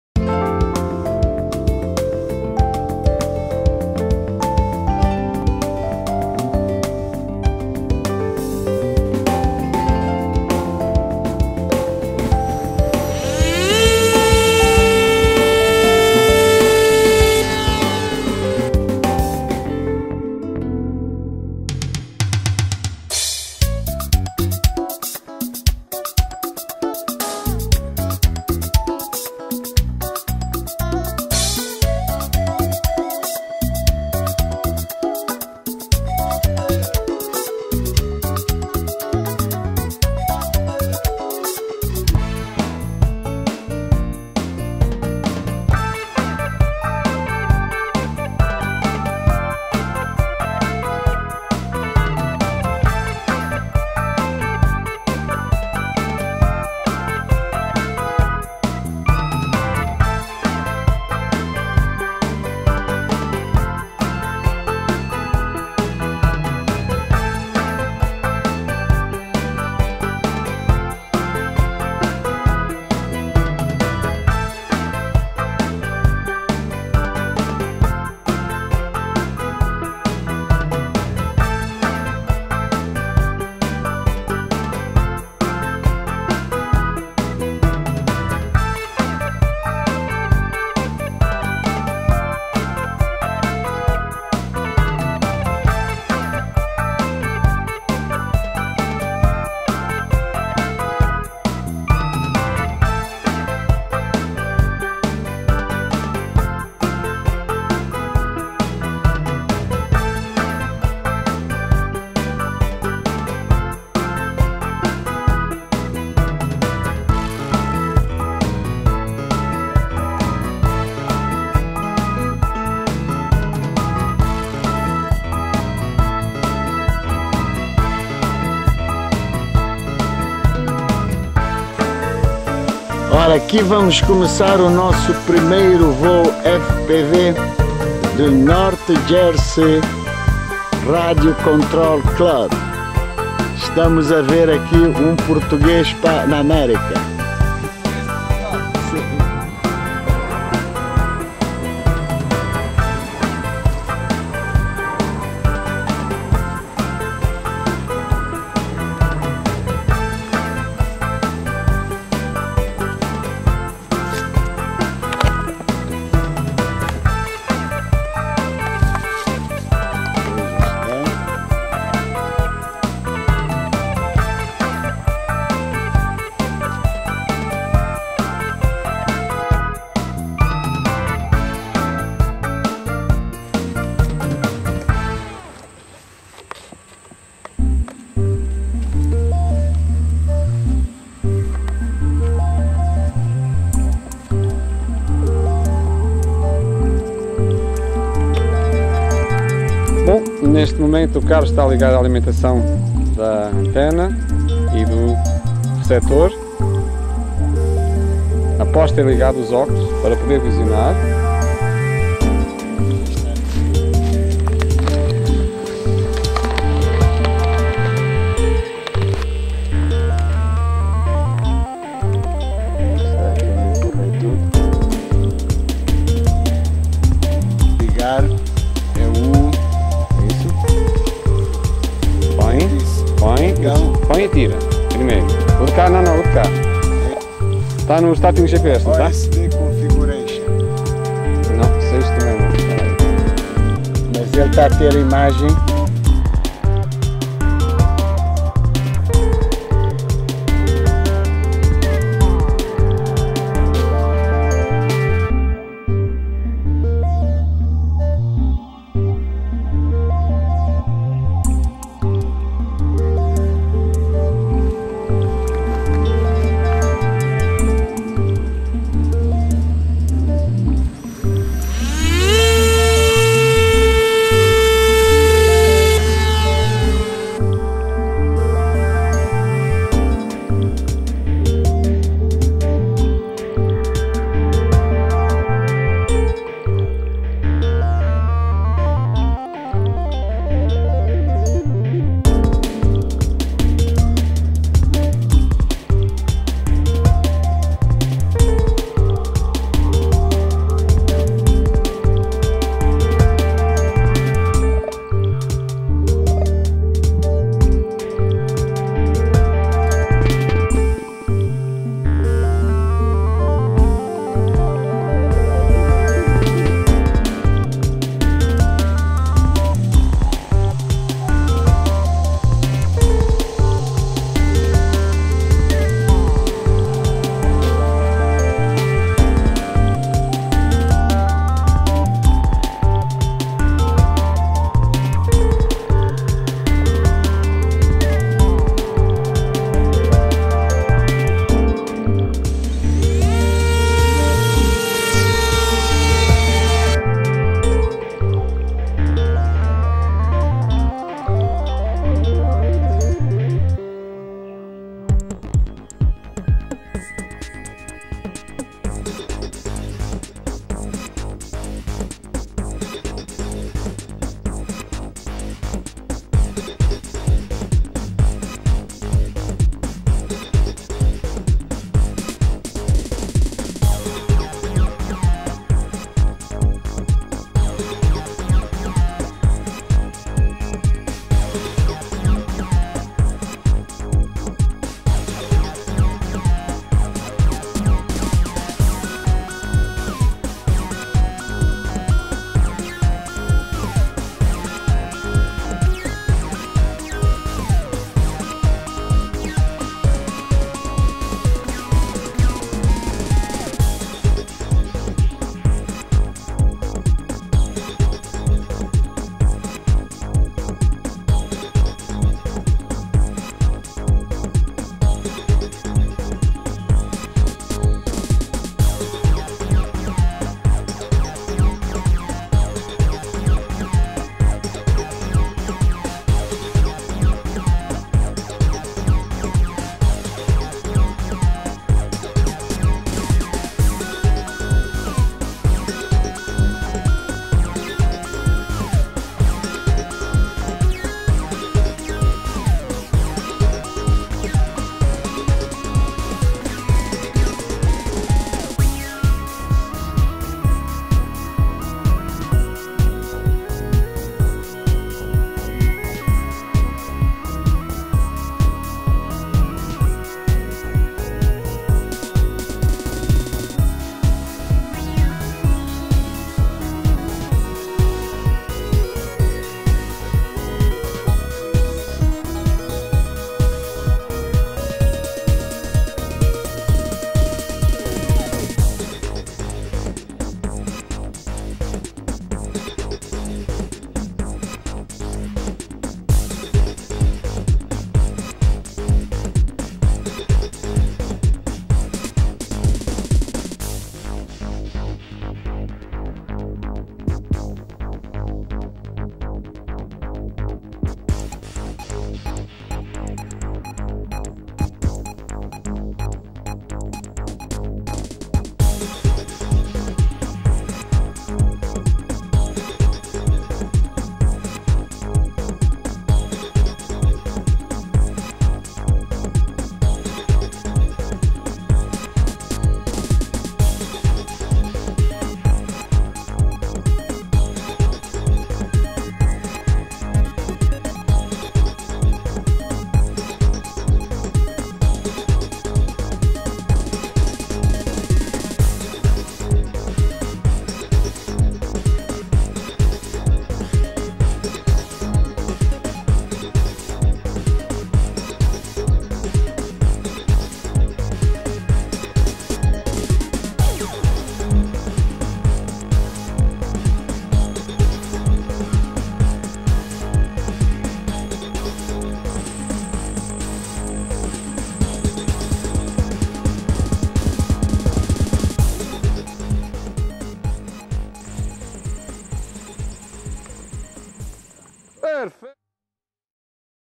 My first FPV... My first flight !
With his five years experience in FPV my friend Aníbal from Portugal, came to give me a hand To me, this is a unique experience ...after that you can hardly not be completely in love with this way of flying!